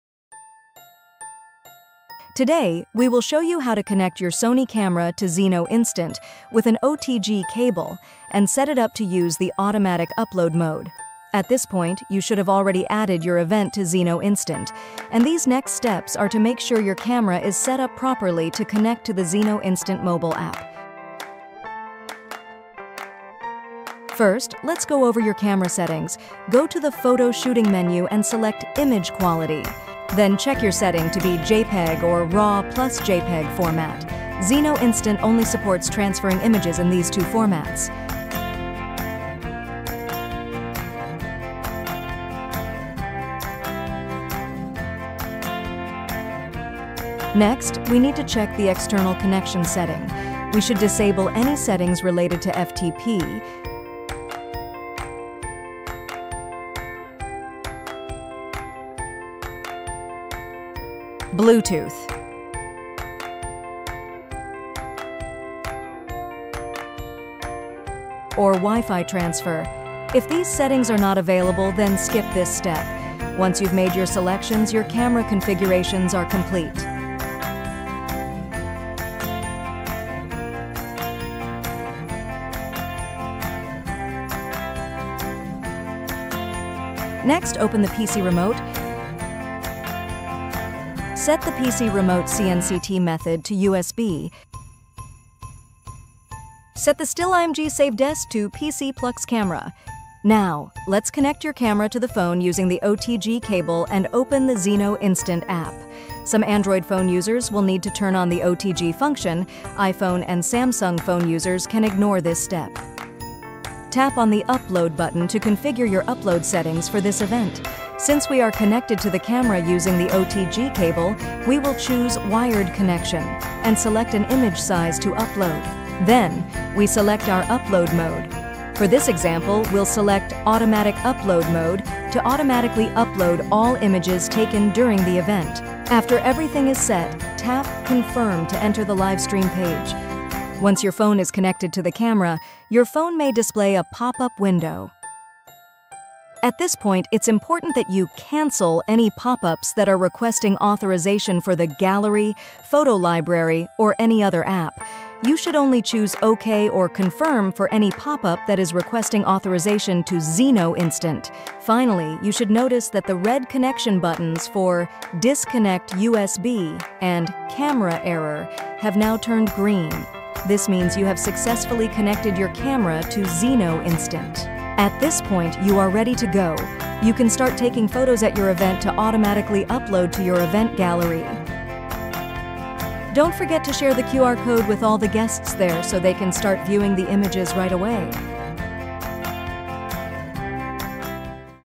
Zno Instant Tutorial: How to Connect Your Sony Camera with a Wired Connection for Automatic Upload
Watch this video to easily learn how to setup your Sony camera to connect to your phone with an OTG cable to use Zno Instant for Automatic Upload. With the help of Zno Instant, you can work more efficiently, enhance your clients' satisfaction and increase your profits.